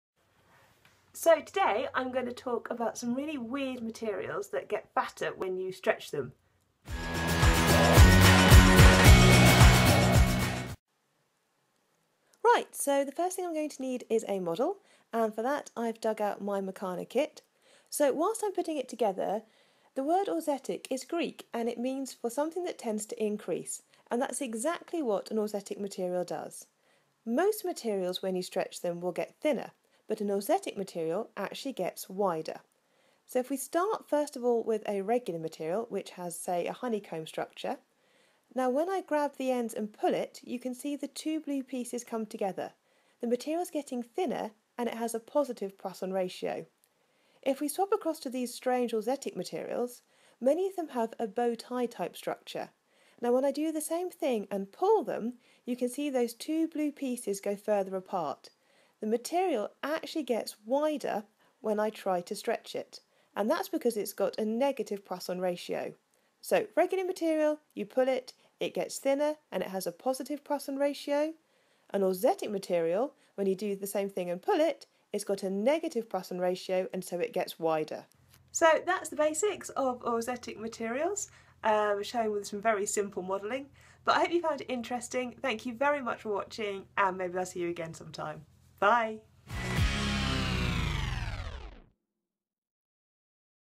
Auxetic Materials
This week a very short video but on some neat auxetic materials which get wider when you stretch them! Thanks as always to @warwickuni for my backdrop artwork :-)